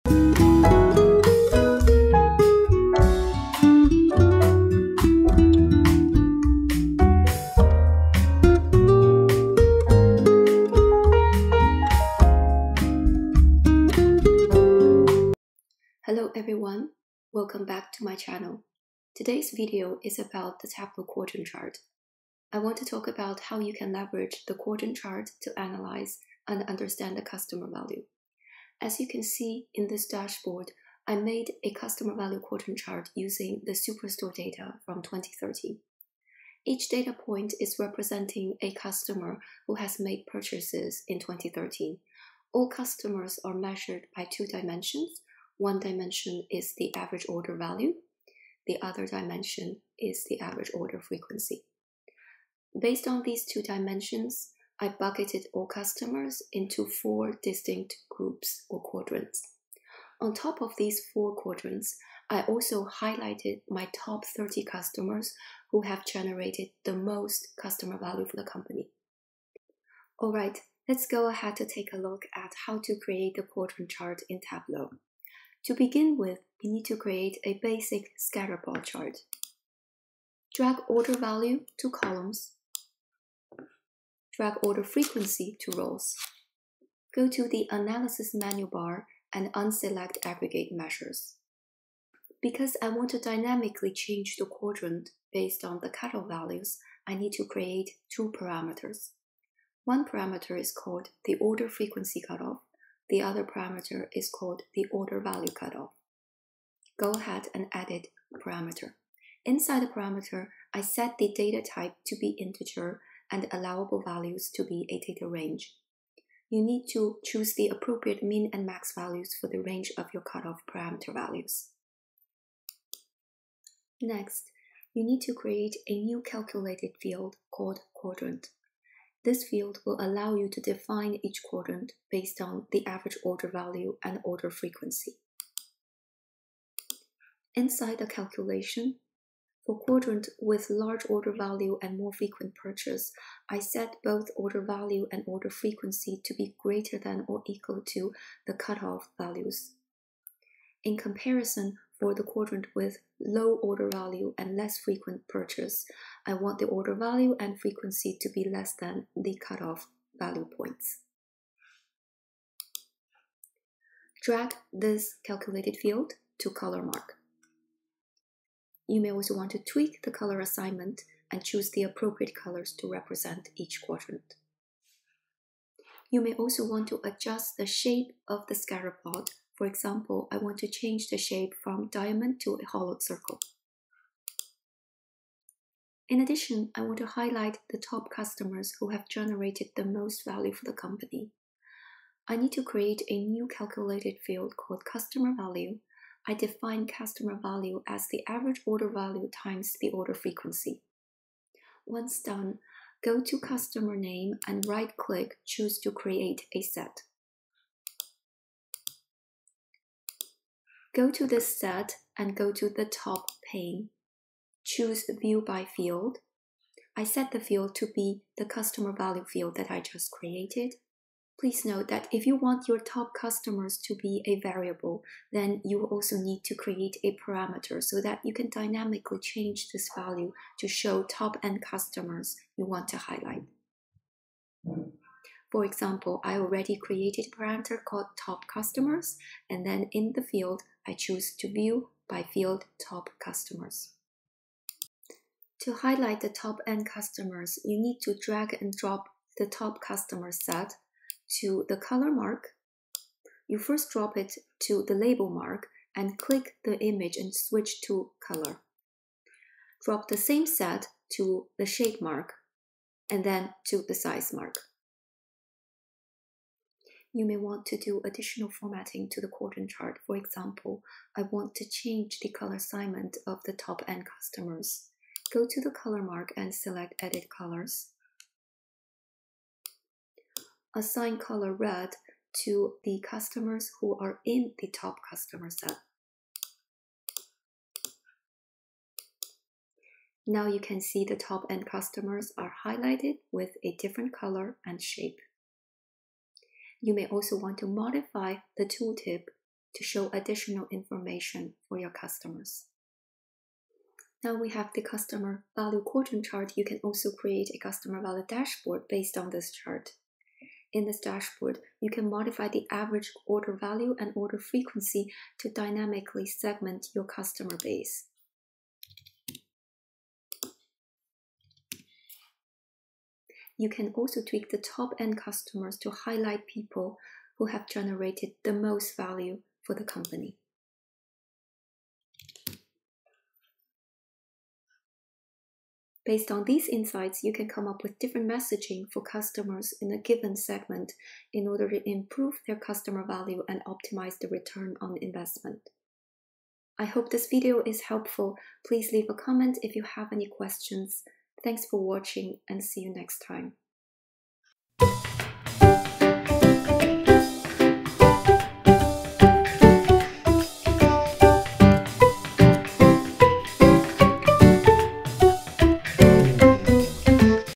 How to Create Quadrant Chart in Tableau
Hello everyone! In this video, I'll show you how to create the customer value quadrant chart in Tableau.